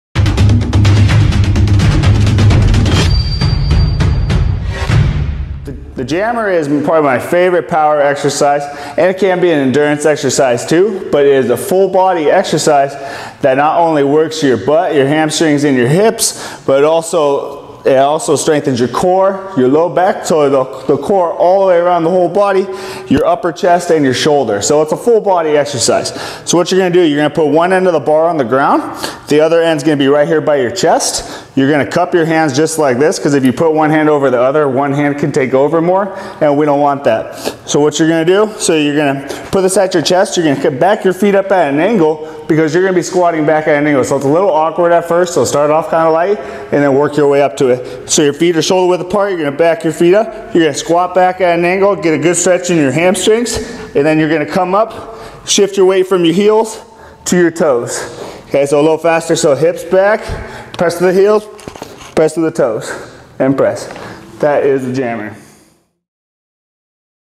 Jammer
3 of 5 Compound Movement Multiplanar Exercises for Elite Level Athletes

How to do it:
1.Grab a barbell
2.Put one end on the ground and grab at the end of the other side of the bar with your hands in a cupped position.
3.While standing tall take a step back with your feet,  your body should be leaning slightly forward.
4.With the bar close to your neck and your feet shoulder width apart, squat back at an angle by pushing your hips back as soon as you 5.bend your knees.
6.Squeeze your shoulder blades and keep your core tight,
7.Lower yourself down until you feel a good stretch in your hamstrings.
8.Once you have reached the bottom, drive through your heels to push yourself up.
9.Once you have just about reached the top, shift your weight to your toes and explode your hand forward.
10.Your body should be fully extended and leaning slightly forward
11.Return the bar back to your chest and repeat

Perform 4 sets of 8